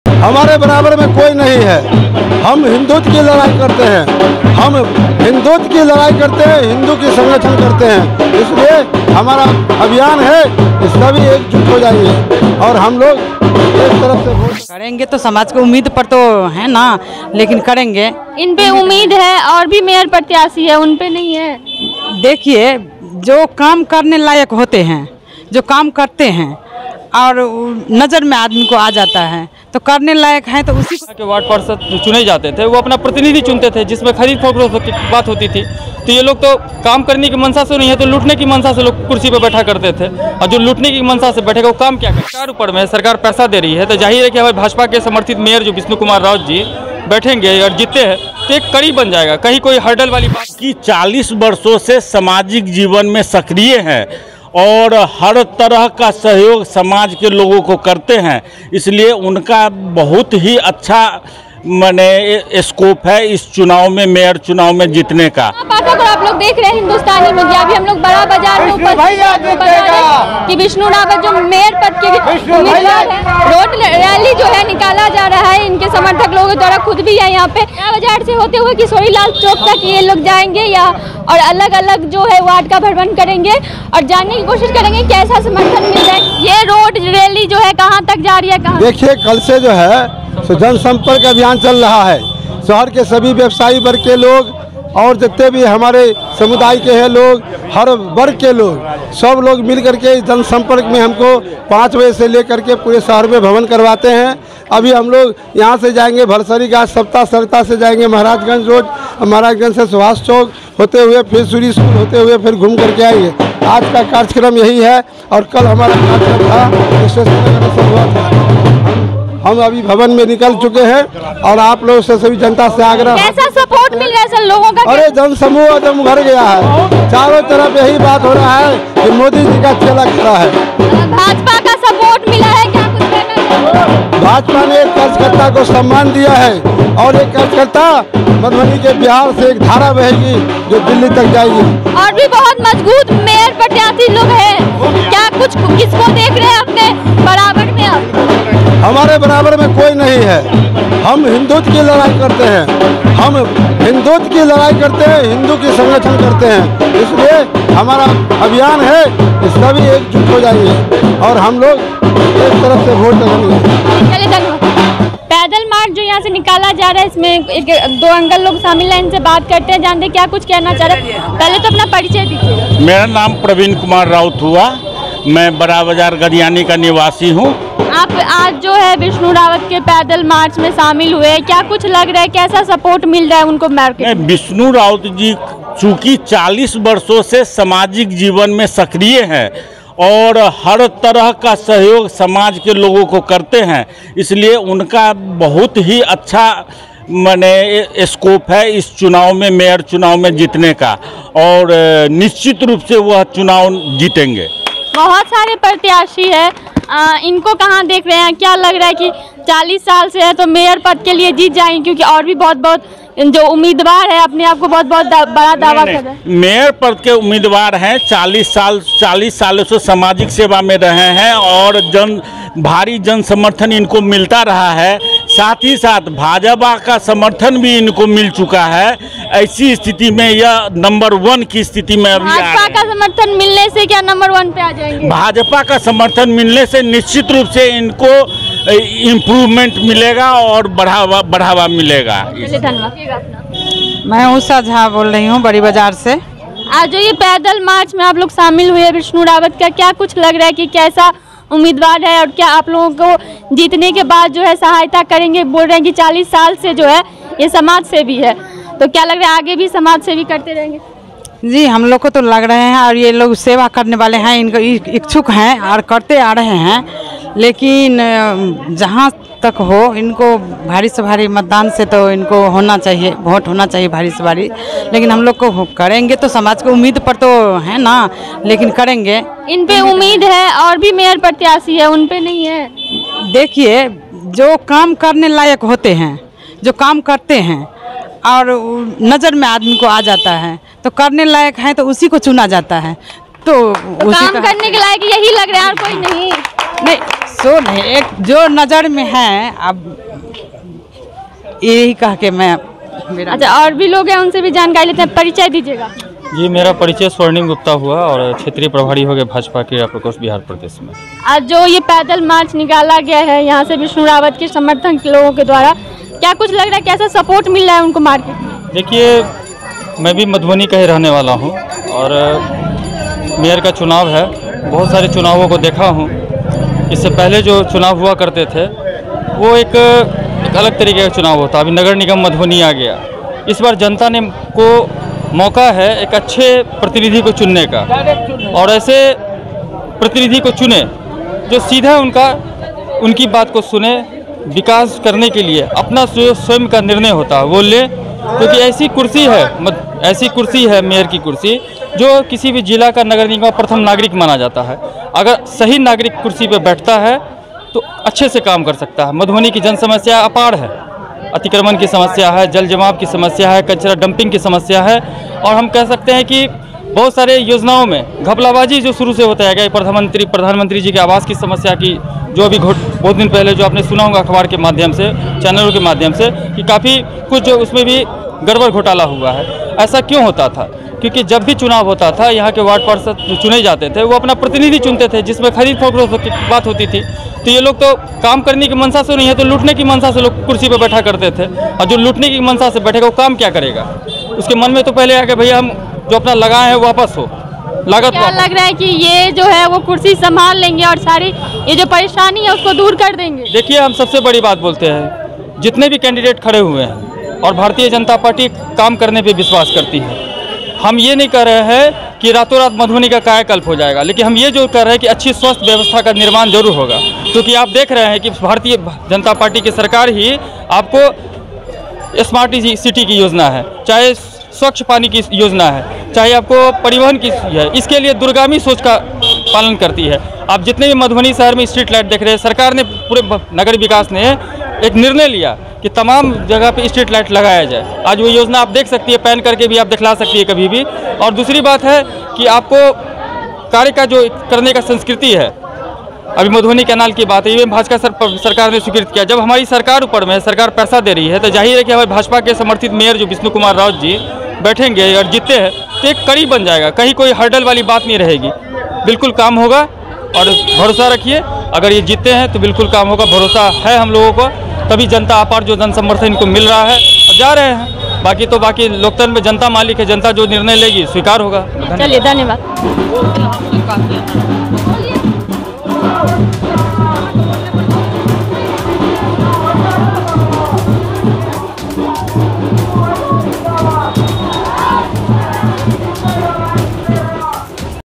BJP समर्थित Mayor प्रत्याशी Vishnu Raut ने निकाला पैदल मार्च, बोले - हिंदुत्व के लिए काम कर रहे
BJP समर्थित Mayor प्रत्याशी Vishnu Raut ने निकाला पैदल मार्च, बोले - हिंदुत्व के लिए काम कर रहे, सबलोग एक हो जाइए...